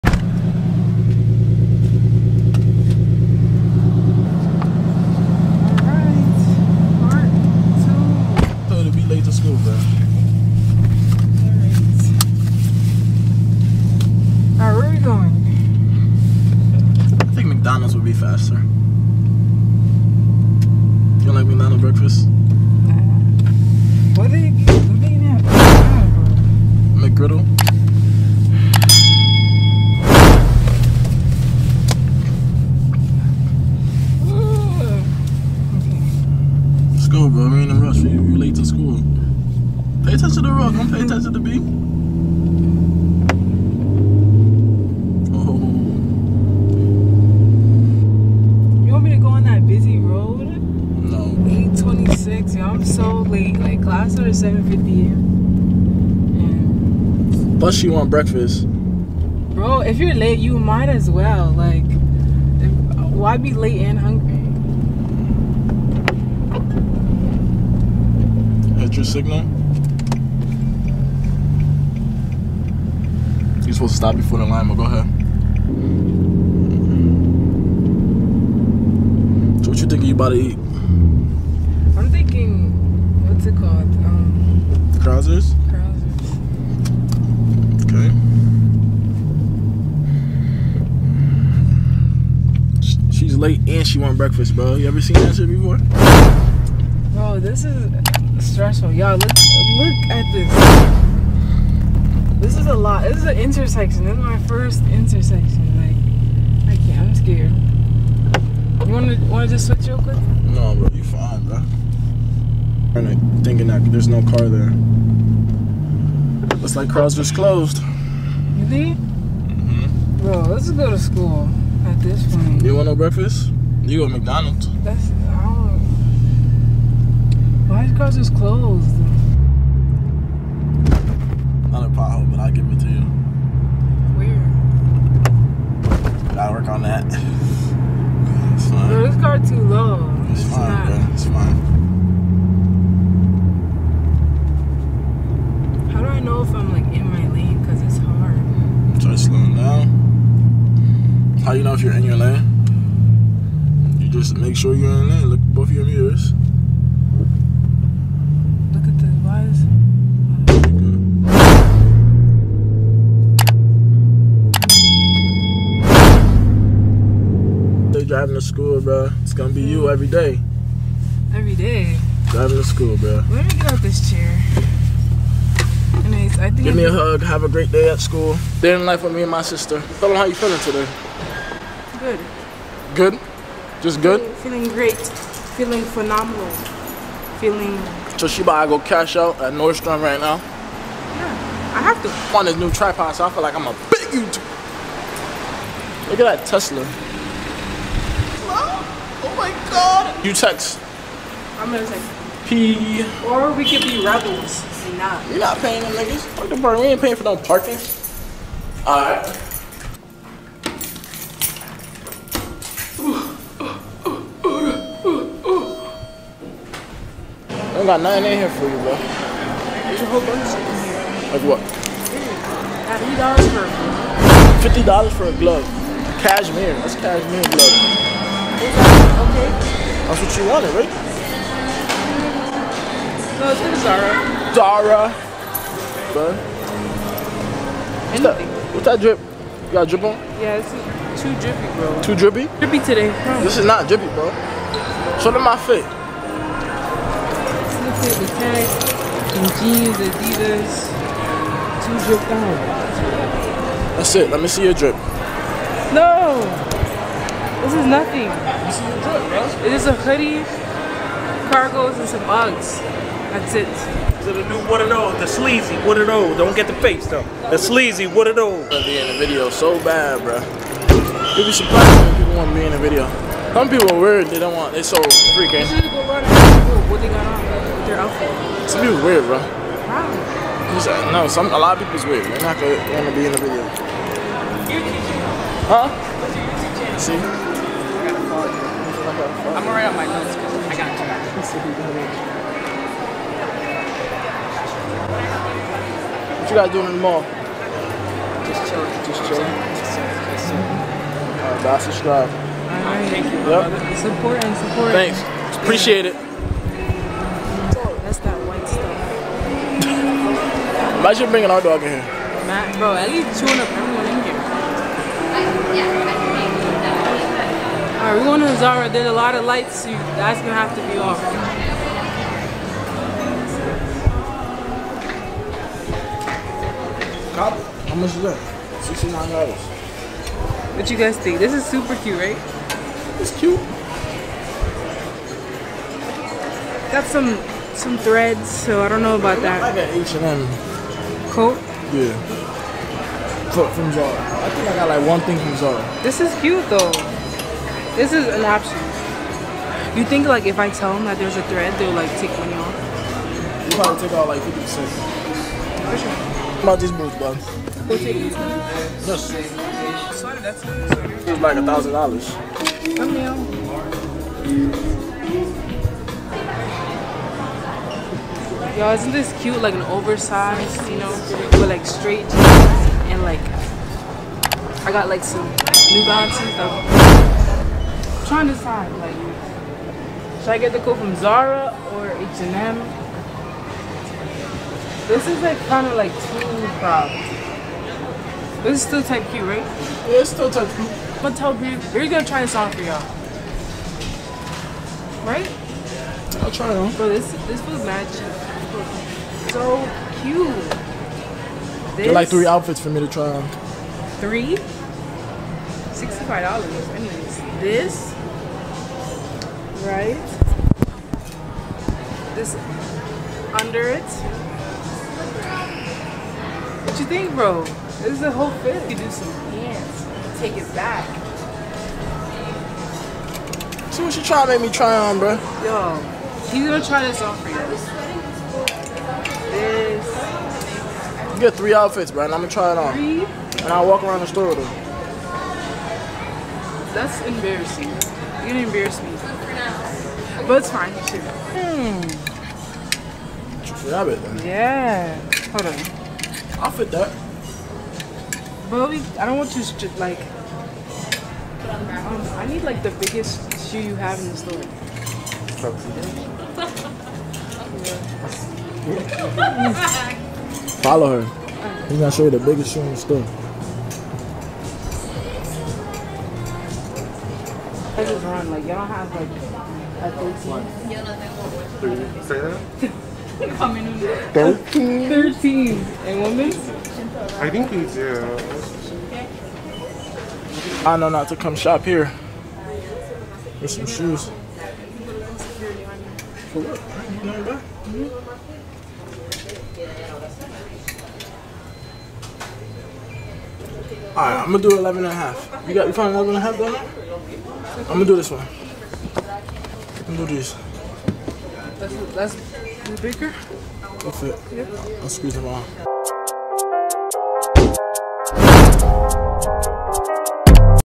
Let My Little Sister drive to school on the First day in my G37😳
Let My 16 Y/o Sister drive to school on the First day !

LMK ANY YOUTUBE IDEAS
MY SOCIAL
SONG USED IN THESE VIDEO I DONT OWN OR HAVE ANY RIGHT TO THE MUSIC
100✅
200✅
500
1000
15k
20k
30k
50k